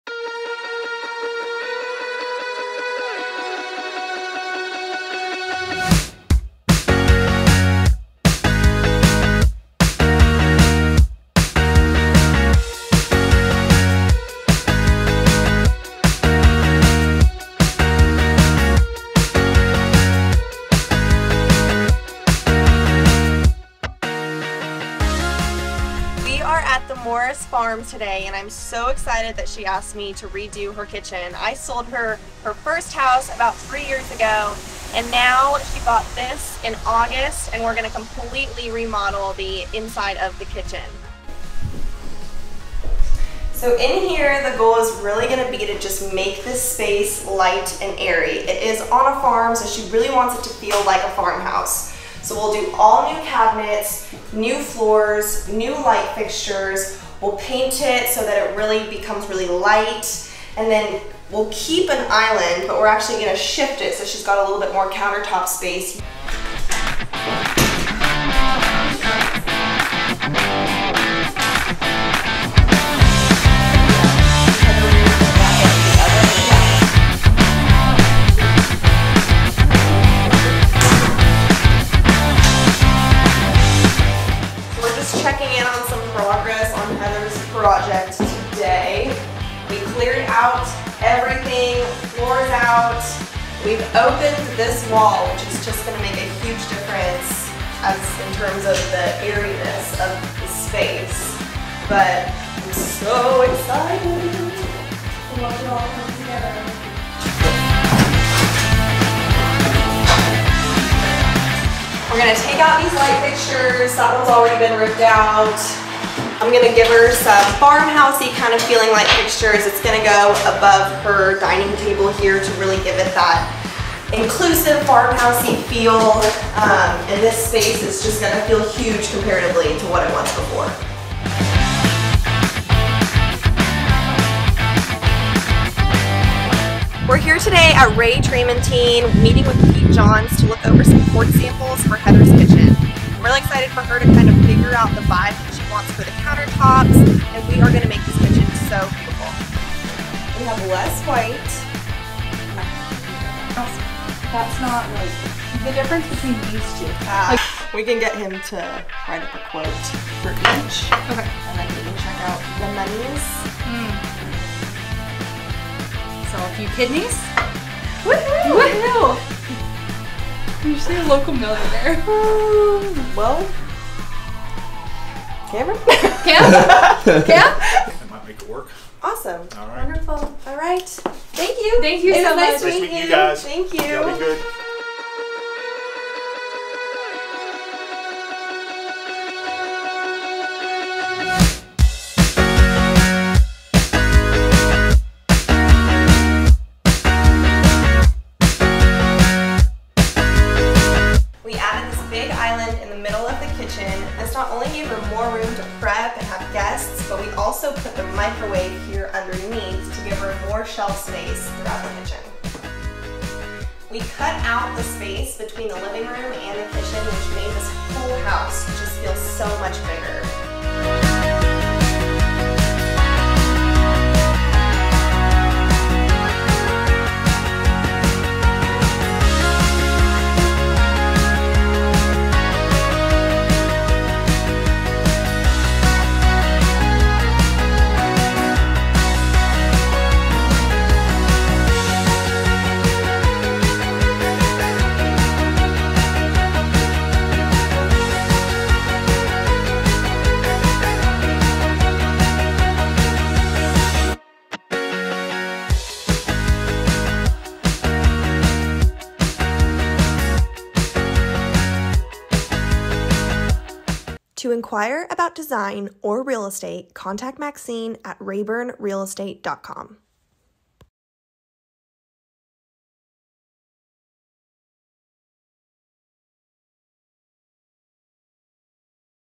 Farmhouse Kitchen MiniReni
I am beyond grateful to have gotten to design Heathers kitchen!
Heather had two requests, farm house feel and a blue island. Everything else she entrusted to me. This project came together thanks to the beautiful work and excellent contracting of PD Designs.